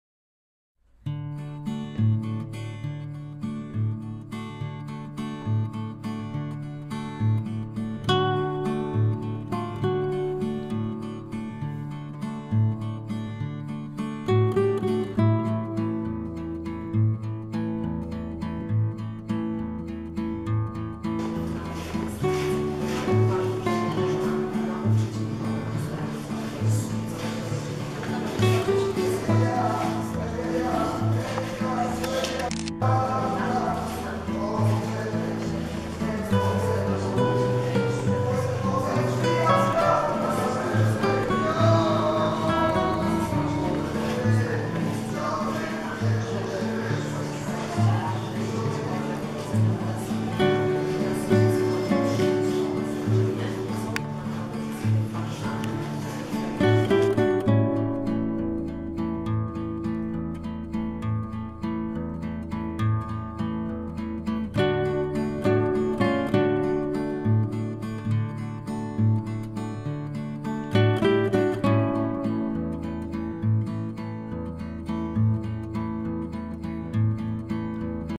2019 02 24 Muzeum Narodowe Wystawa Pabla Picassy cz 14
Muzeum Narodowe w Warszawie - w oczach Romana / National Museum in Warsaw - in Roman's eyes / Musée National à Varsovie - dans les yeux de Roman
Wystawa Pabla Picassy / The Pablo Picassa exhibition / L'exposition de Pablo Picassa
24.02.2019 po / after / après 10.00